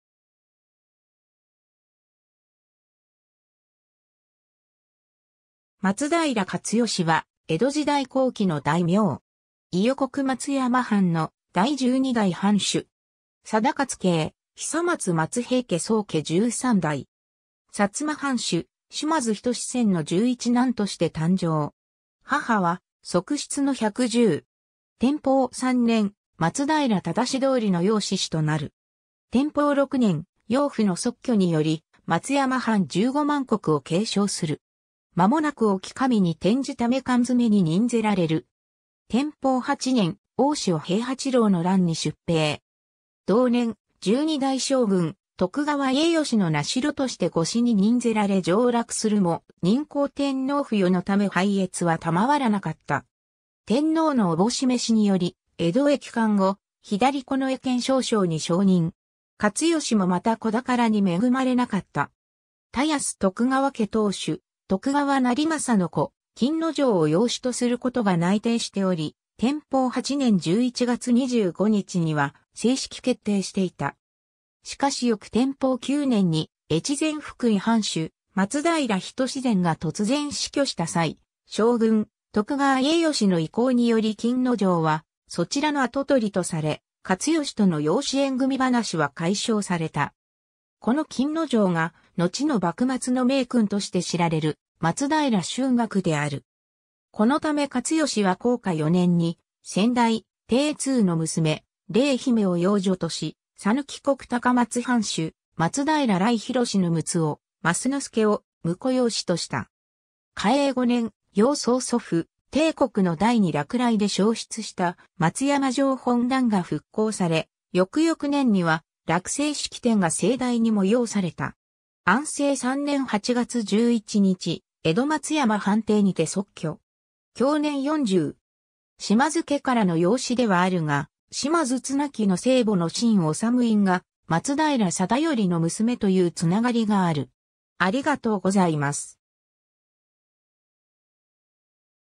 松平勝善
松平 勝善（まつだいら かつよし）は、江戸時代後期の大名。
伊予国松山藩の第12代藩主。
定勝系久松松平家宗家13代。
 薩摩藩主・島津斉宣の11男として誕生。
母は側室の百十（真如院殿）。
天保3年（1832年）、松平定通の養嗣子となる。
天保6年（1835年）、養父の卒去により松山藩15万石を継承する。
間もなく隠岐守に転じ溜間詰に任ぜられる。
天保8年（1837年）、大塩平八郎の乱に出兵。
同年、12代将軍・徳川家慶の名代として御使に任ぜられ上洛するも、仁孝天皇不予のため拝謁は賜らなかった。
天皇の思し召しにより、江戸へ帰館後、左近衛権少将に昇任。
 勝善もまた子宝に恵まれなかった。
田安徳川家当主・徳川斉匡の子・錦之丞を養子とすることが内定しており、天保8年（1837年）11月25日には正式決定していた。
しかし翌天保9年（1838年）に越前福井藩主・松平斉善が突然死去した際、将軍・徳川家慶の意向により錦之丞はそちらの跡取りとされ、勝善との養子縁組話は解消された。
この錦之丞がのちの幕末の名君として知られる松平春嶽である。
このため勝善は弘化4年（1847年）に先代・定通の娘・令姫を養女とし、讃岐国高松藩主・松平頼恕の六男・増之助（のち勝成）を婿養子とした。
 嘉永5年（1852年）、養曾祖父・定国の代に落雷で焼失した松山城本壇が復興され、翌々年には落成式典が盛大に催された。
安政3年（1856年）8月11日（8月10日とも言われている）、江戸松山藩邸にて卒去。
享年40。
 島津家からの養子ではあるが、島津綱貴（斉宣から5世代前の先祖）の生母の真修院が松平定頼（久松松平家3代）の娘というつながりがある。